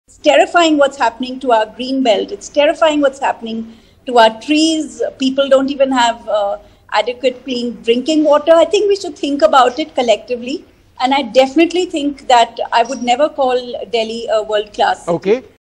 What Will Arvind Kejriwal Present At Singapore Conference? Activist Asks | Left, Right & Centre
It is not enough to approve of some projects, says Bharti Chaturvedi, founder and director, Chintan Environmental Research and Action Group. She had questioned Delhi Chief Minister Arvind Kejriwal's Singapore trip, asking what would he even present at the conference given the state of Delhi's environment.